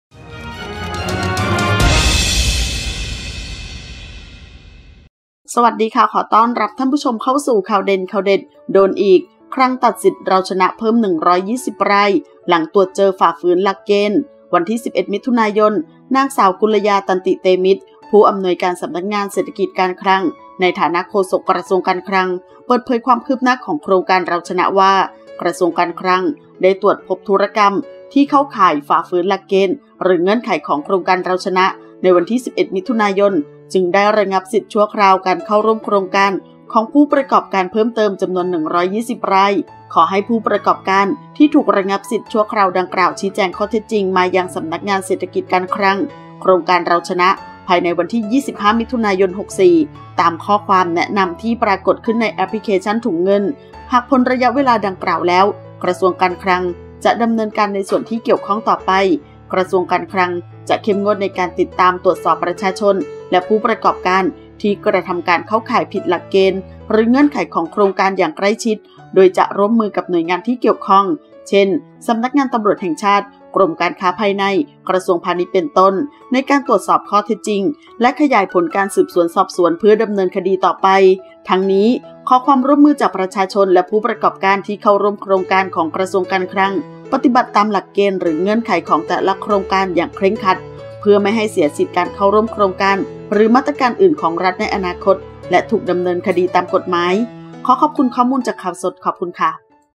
โดนอีก! คลังตัดสิทธิ์ “เราชนะ” เพิ่ม 120 ราย หลังตรวจเจอฝ่าฝืนหลักเกณฑ์
ขอบคุณที่มา :
ขอขอบคุณข้อมูลจาก: